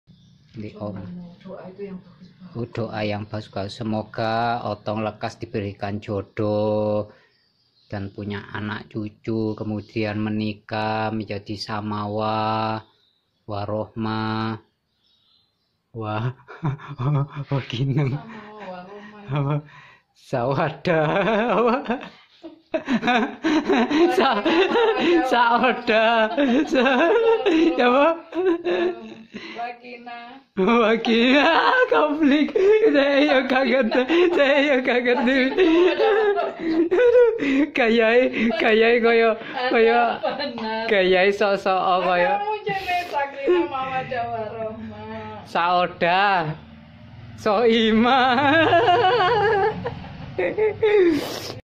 SITKOM : DOA BAIK
yuk doain sultan Otong segera nikah